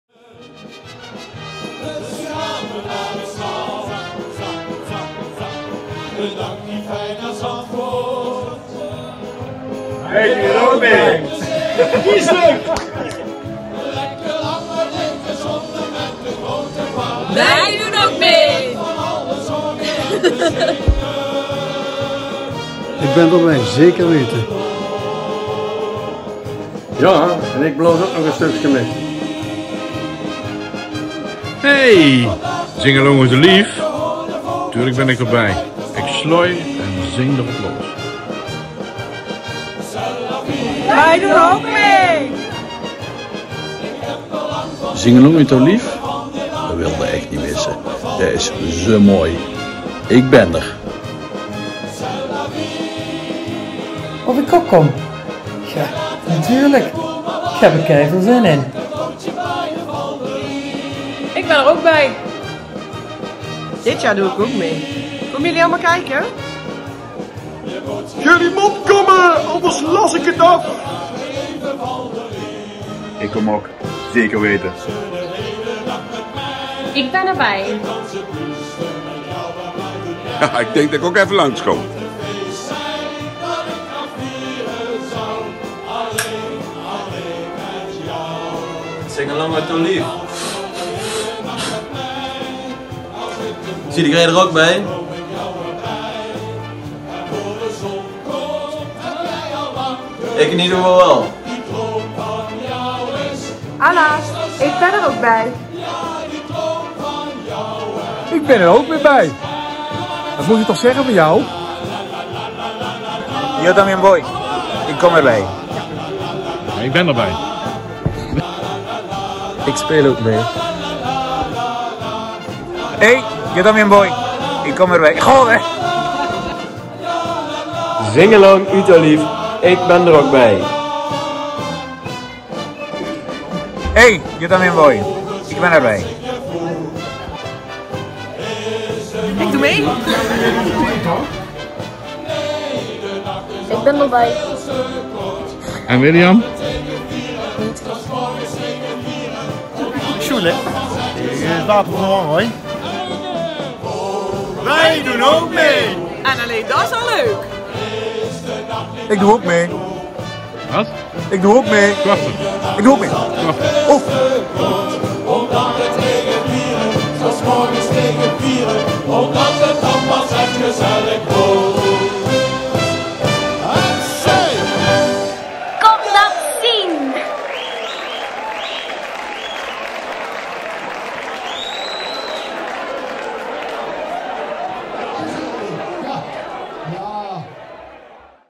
Zing A Long Uut Ouw Lief Promo
Zing A Long Uut Ouw Lief, de carnavalsproms van St Tunnis op zaterdag 18 februari 2023 met lokale artiesten! Komt Dat Zien!